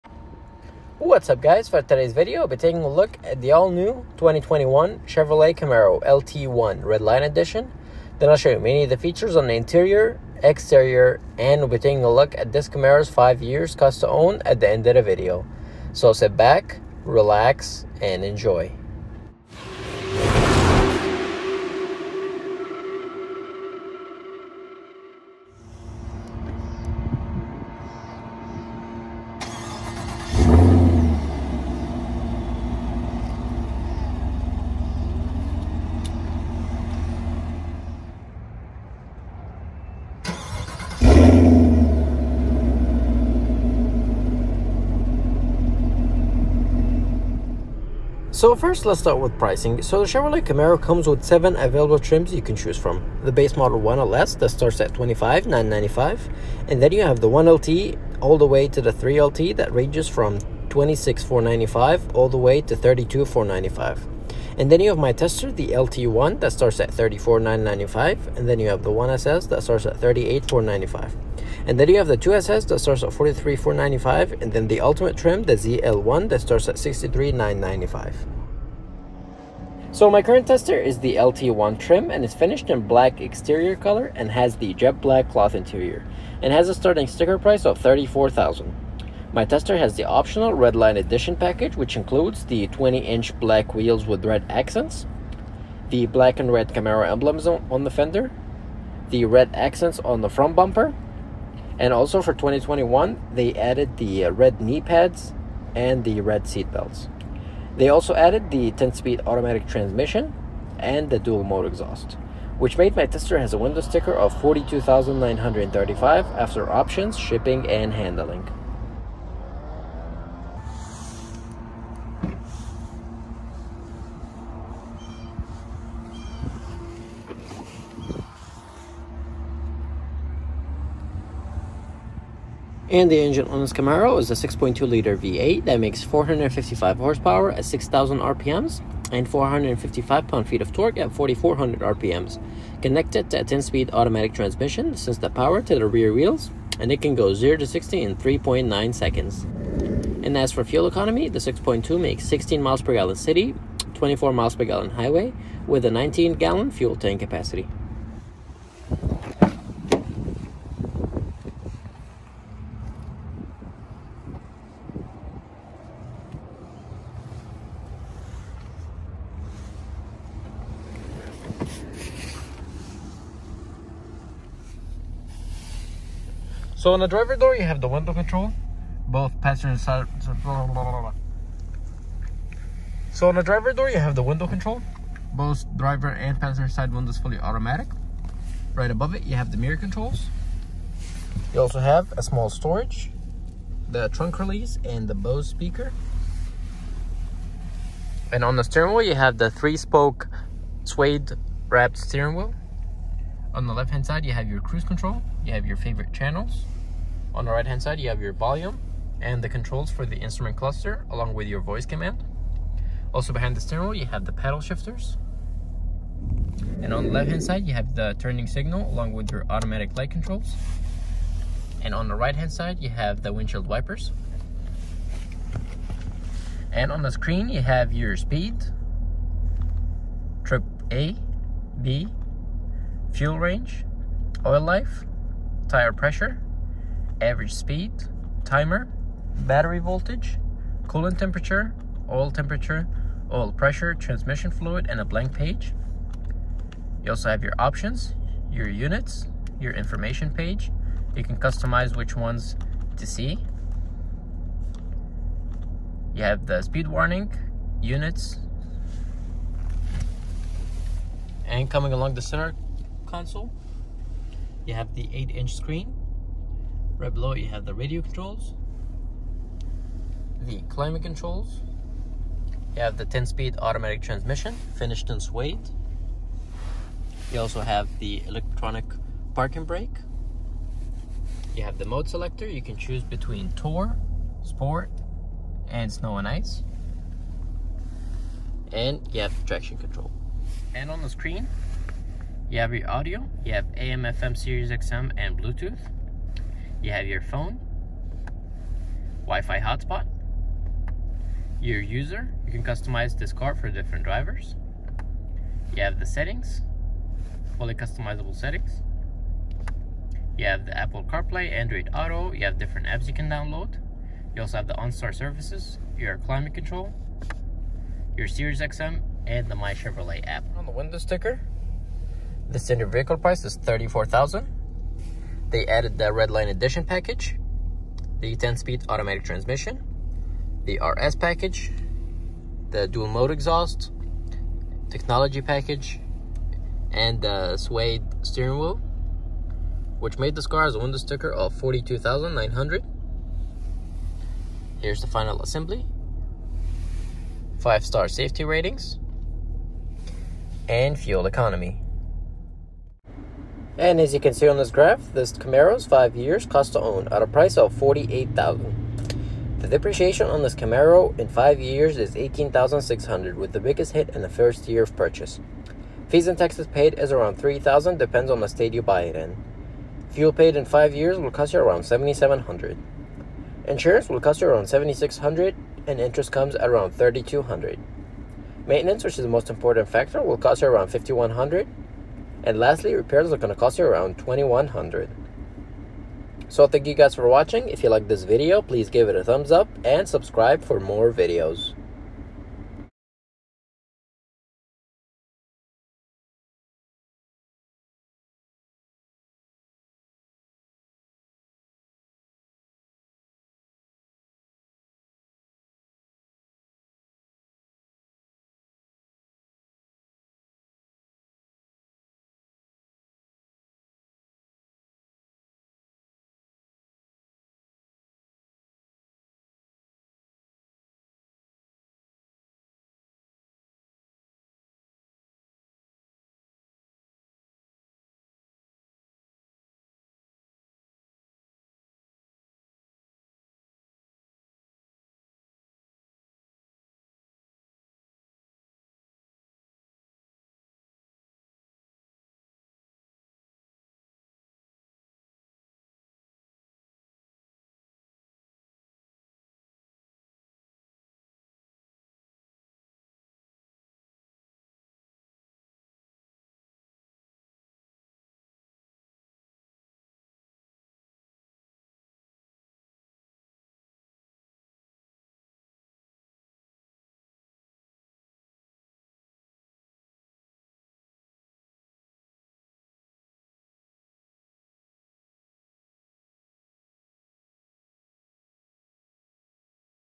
2021 Chevrolet Camaro LT1 Redline Edition: Start up & Review
Specs:

INTERIOR
Front head room 39 "
Rear head room 34 "
Front shoulder room 55 "
Rear shoulder room 50 "
Front hip room 55 "
Rear hip room 46 "
Front leg room 43.9 "
Rear leg room 29.9 "
Luggage capacity 9.1 Cu.Ft.
Maximum cargo capacity 9.1 Cu.Ft.
Standard seating 4
EXTERIOR
Length 188.3 "
Body width 74.7 "
Body height 53.1 "
Wheelbase 110.7 "
Curb 3,619 Lbs.
FUEL
Fuel tank capacity 19.0 Gal.
EPA mileage estimates 16 City / 24 Hwy
PERFORMANCE
Base engine size 6.2 Liters
Base engine type V-8
Horsepower 455 Hp
Horsepower rpm 6,000
Torque 455 Lb-Ft.
Torque rpm 4,400
Drive type Rear-Wheel